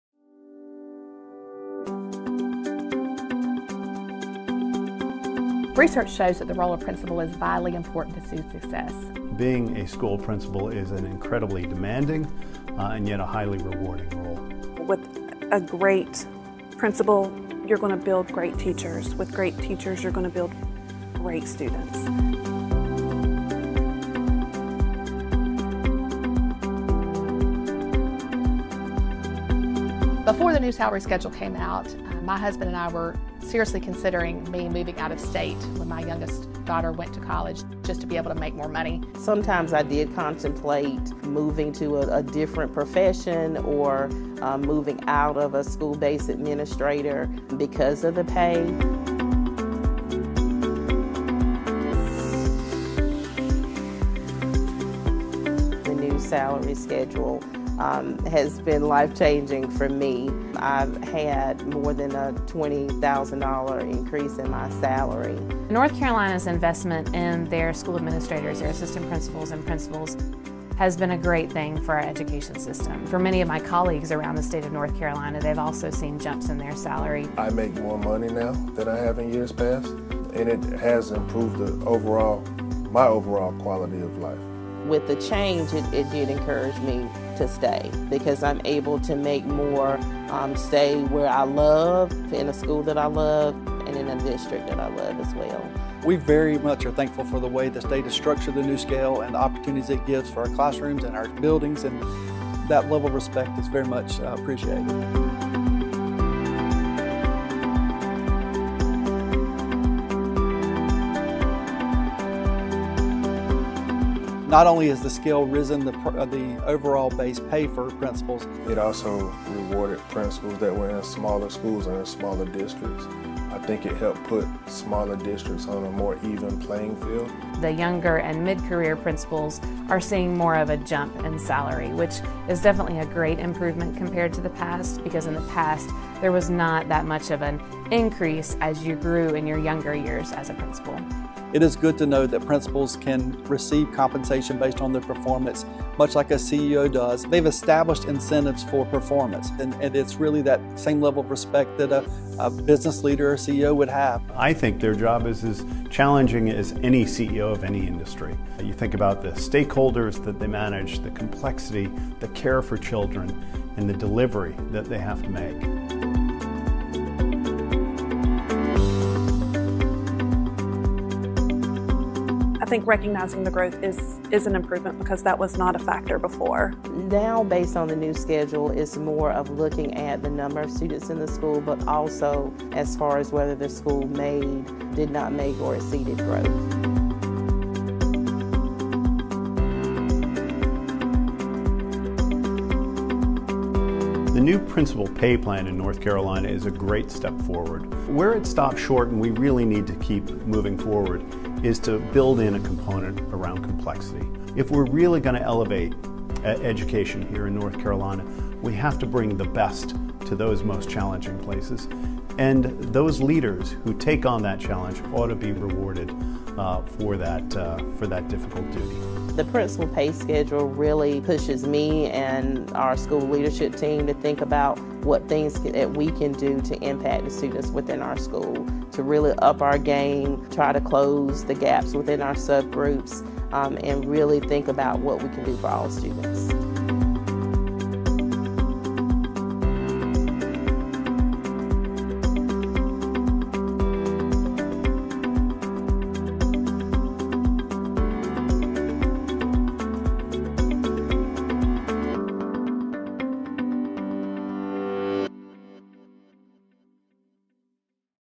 Principal Pay in North Carolina: A Closer Look
Principals are the superheroes of our public schools. And yet, for years, North Carolina ranked near the bottom nationally in principal pay. So in 2017, NC invested in principal salaries - while also transforming the way principals are paid.